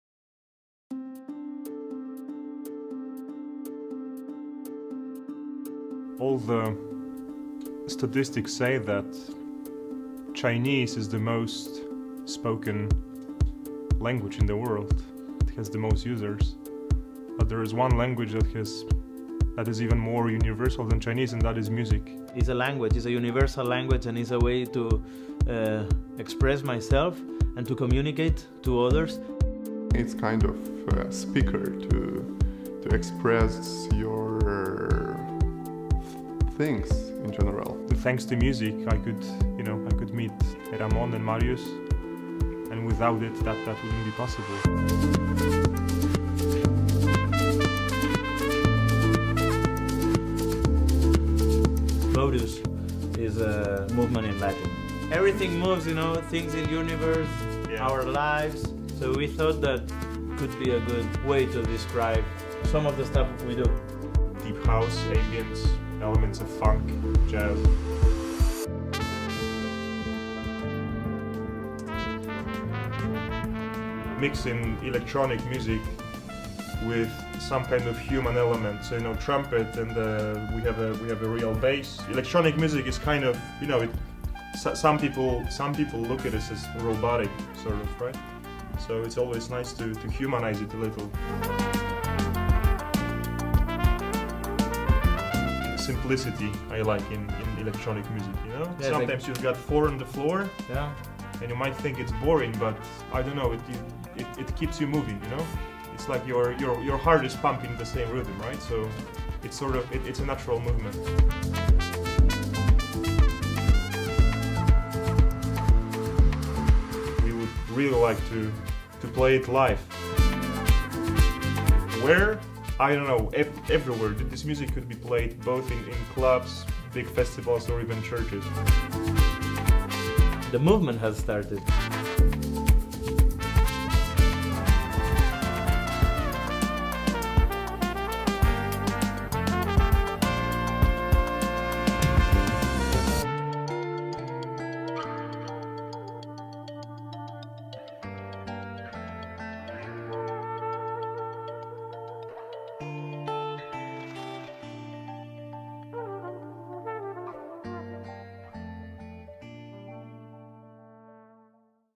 MØTUS | Barcelona - Trailer
We are Greg, Mariusz and Ramon. We love to produce, to play and to create different moods with music.

Discover our new project of deep house mixed with electronic music elements, jazz and ambience music. We recorded 2  singles in Barcelona, at Medusa Studio and they will be available really soon on Spotify and other platforms.

Møtus is:
 - Bass and synths: Mariusz Wiater:
 - Keyboards and synths: Greg Mischyshyn
 - Trumpet, flugelhorn and synths: Ramon Figueras Alsius

Video by El Cocu
Music produced by Møtus Music Lab
Recorded mixed and mastered at Medusa Studio by Juanjo Alba